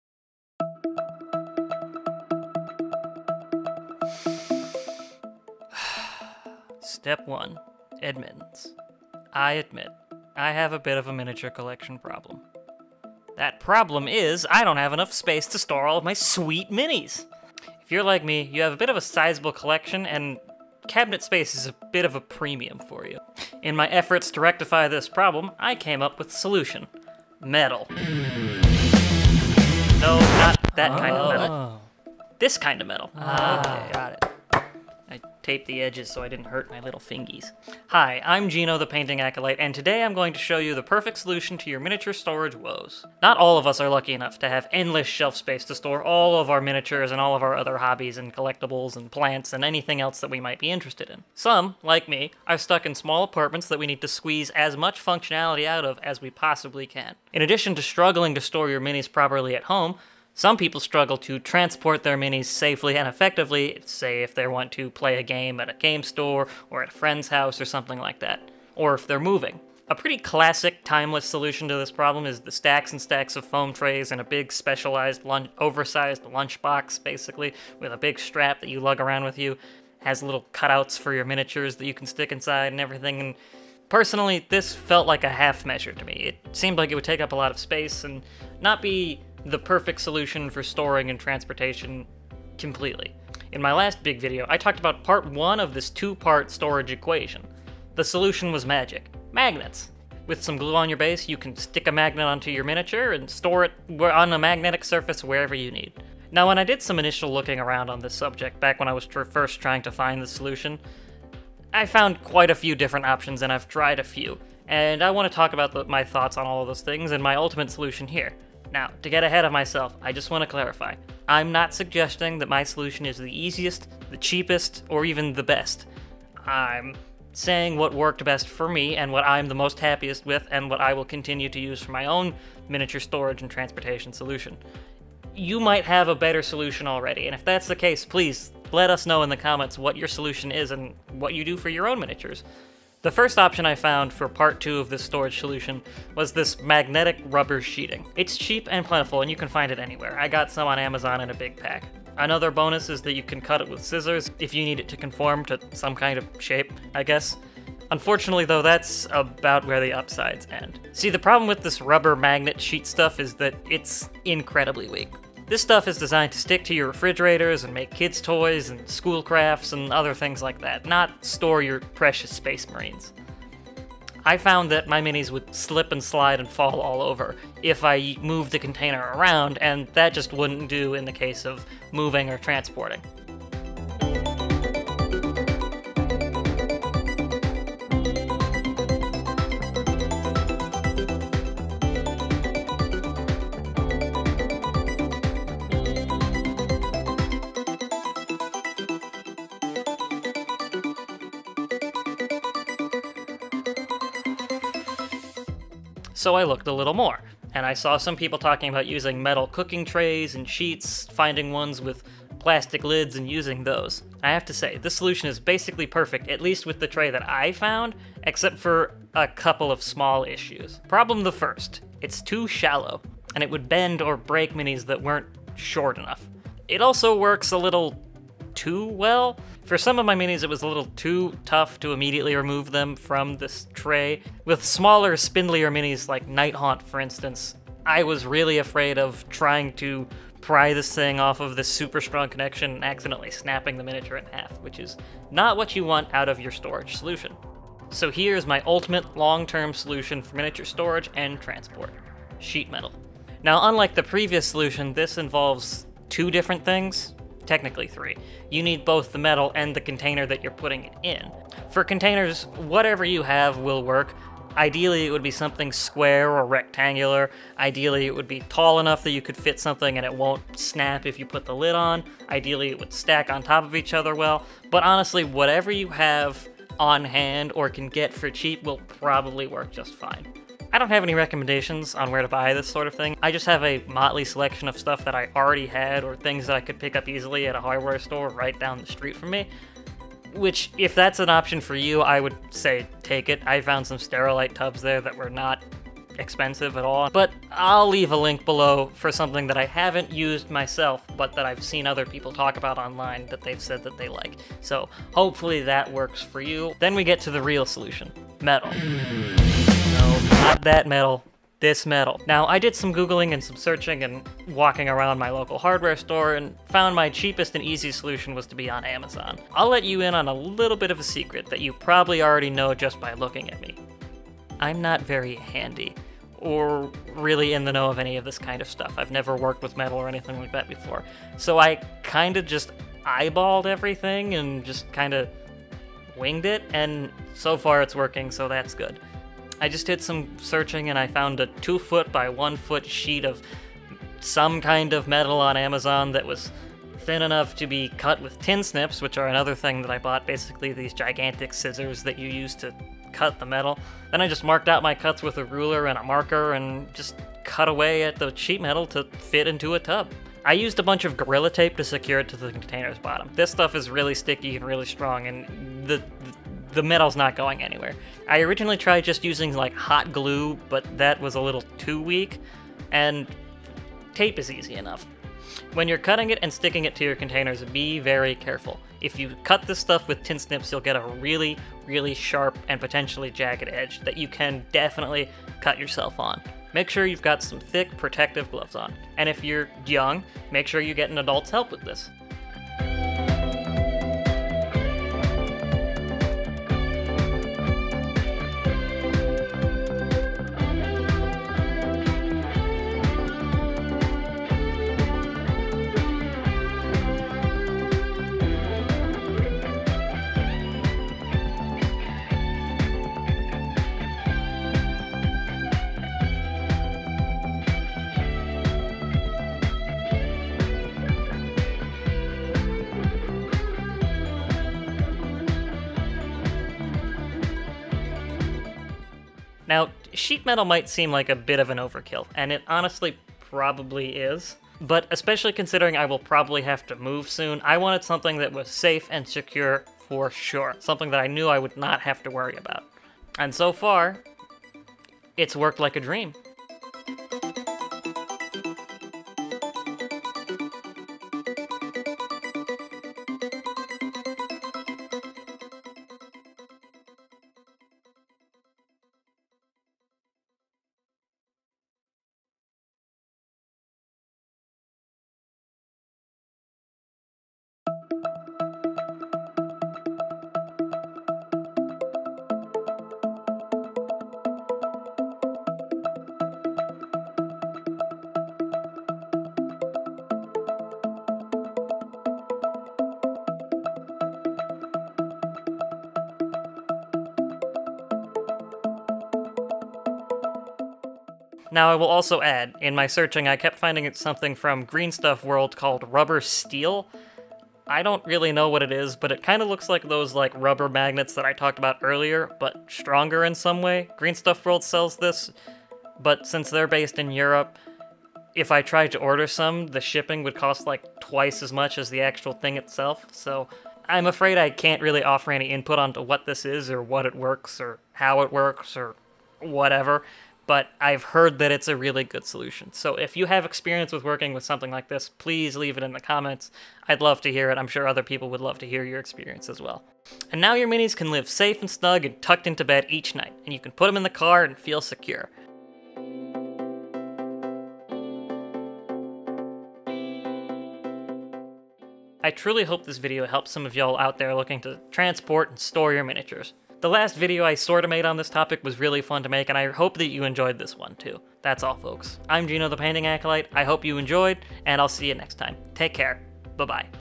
The SECRET to How I Store and Transport my Minis
Lots of folk struggle to store and/or transport their miniatures. Today, I show you the secret weapon I use to solve all my storage woes!

The secret... is metal!

The exact listing I got my sheet metal from has since disappeared. However - sourcing from your local hardware store should be easy. Alternatively, I’ve been able to find perfect sheet metal for cheap on eBay by literally just searching sheet metal. Your results will vary certainly - but it shouldn’t be expensive and it should be easy to find somewhere!

Link to the plastic tubs I've heard people use

If you have any other recommendations for storage/transport, leave them in the comments below!